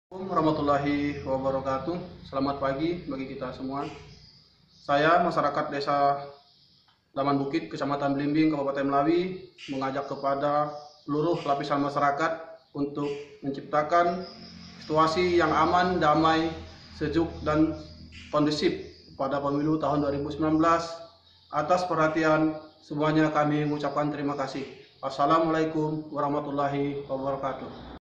Warga Masyarakat Kec. Belimbing, Deklarasi Pemilu 2019
Warga Masyarakat Kec. Belimbing, Deklarasi Pemilu 2019 dengan mengajak segenap masyarakat Kab. Melawi untuk bersama sama menciptakan situasi aman damai dan sejuk di Pemilu 2019